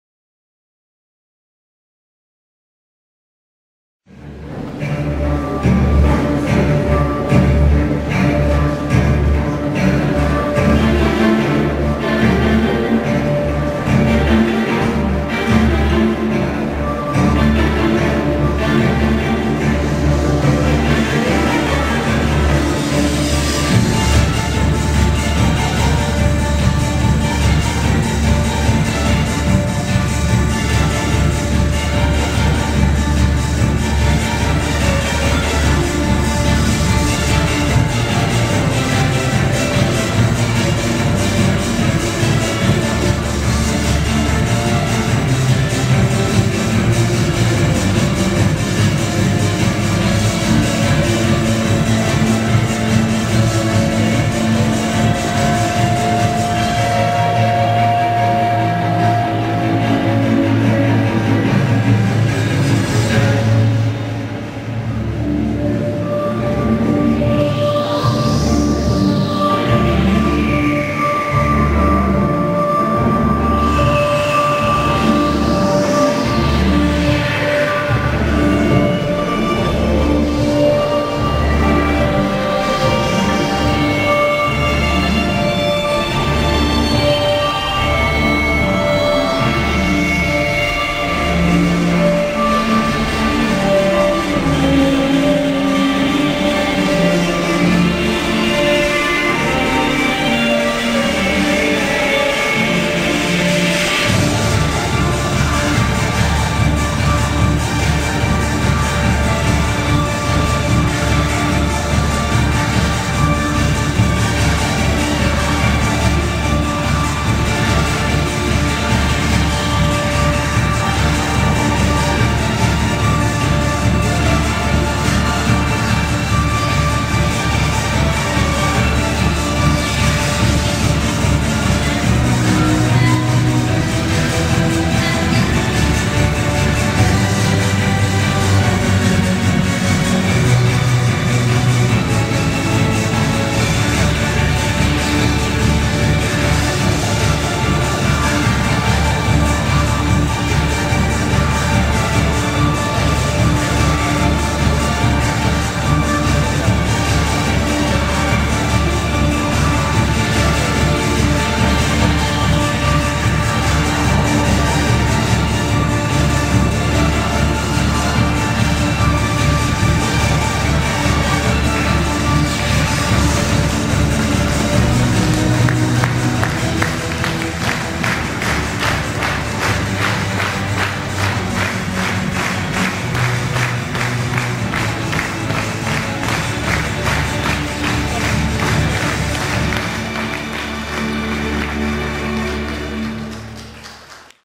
Impresja Olimpijska - zespół Dynamik
Plebiscyt na 10 Najlepszych Uczniów-Sportowców Województwa Opolskiego za rok 2014 szkół podstawowych i gimnazjalnych w Publicznej Szkole Podstawowej nr 3 w Grodkowie